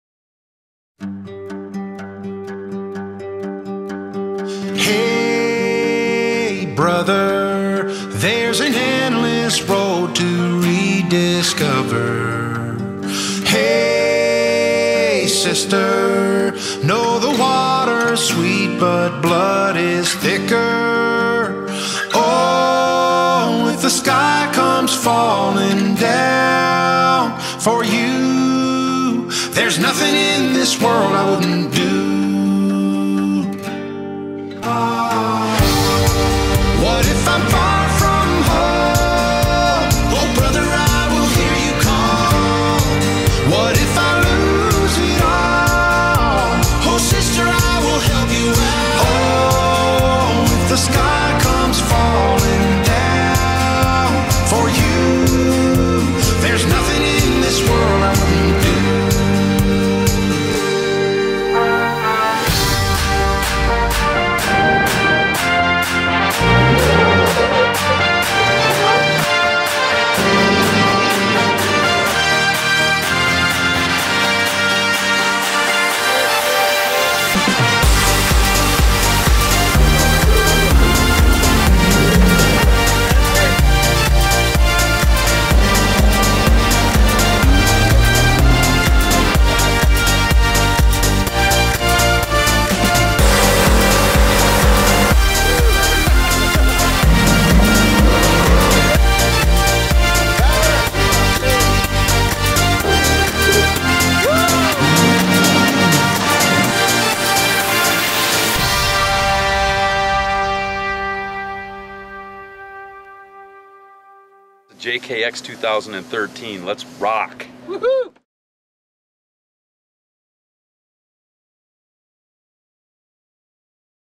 WILD WILD WEST : The 2013 JK-Experience Trailer / a WAYALIFE Presentation
Starting in Hurricane, Utah, and ending up in South Lake Tahoe, California, the 2013 Off Road Evolution JK-Experience presented by Nitto Tire was designed to be one of the most demanding excursions to date. Covering almost 1,000 miles of America's wild west, in just seven days and approximately half of them being all off road, this trip would prove to be an adventure of a lifetime that would push our components to the limit, and those of our gas tanks too. A full feature length film highlighting all the fun we had will be coming your way soon!

More than just a Jeep, it's a way of life!

For those of you on mobile devices, please visit our V i m e o channel as you will not be able to view this video here on YouTube.